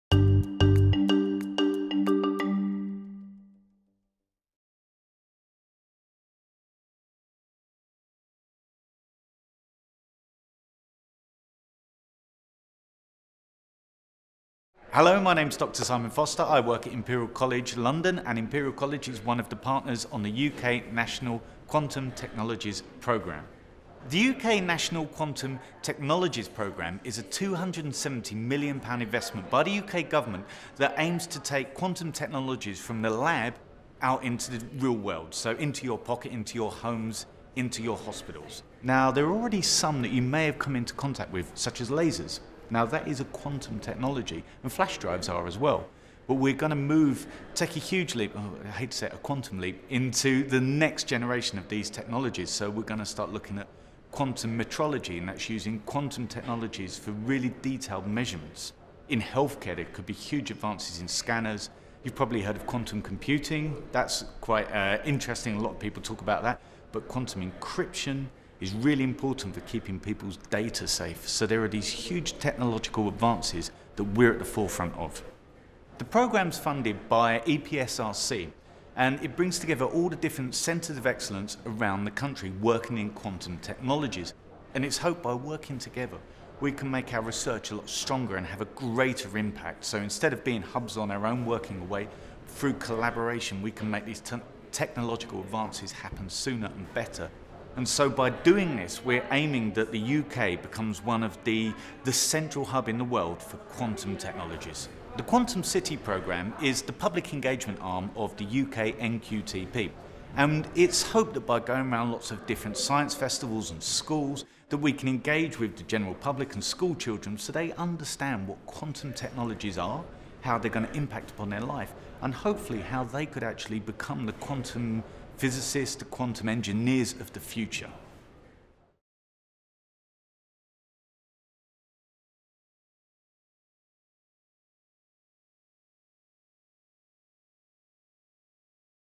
Welcome to Quantum City!
Short video introducing the Quantum City public engagement initiative, its partners, funding, aims and key messages.

Quantum City is a joint public engagement initiative by many stakeholders of the UK National Quantum Technologies Programme. It brings together researchers from the UK Quantum Technology Hubs, Centres for Doctoral Training and the National Physical Laboratory.

Through a joint communications and impact evaluation plan, a series of science festival demonstrations, strong online presence and a coordinated social media strategy, participating partners hope to instill an understanding of the benefits of quantum technologies, showcase the UK’s expertise in this area, and inspire young audiences to become the next generation of quantum technologists.

Filmed by Digifish at a Royal Institution (London) outreach event in November 2019.